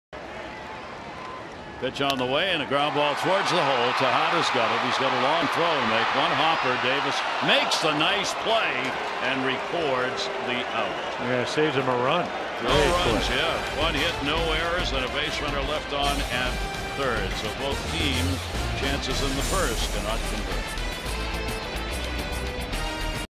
BAL@TEX: Tejada makes a nice play to get Beltre out
Ruben Tejada ranges in the hole to make a backhanded stop and fires to first base to nab Adrian Beltre in the bottom of the 1st inning